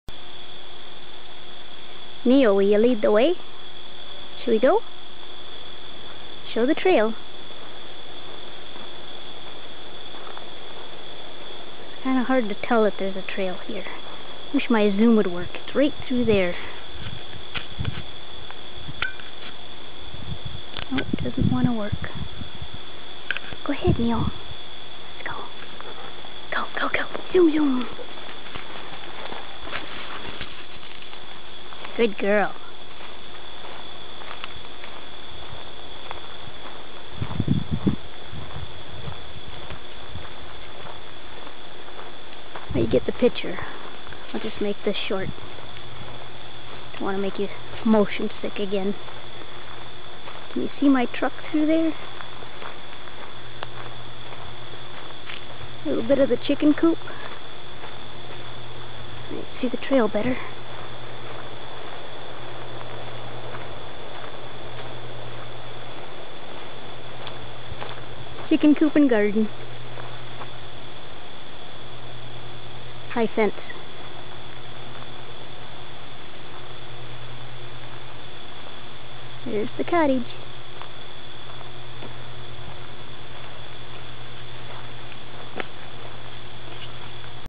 Camping2
Going from Tent to cabin through the pines with Neo.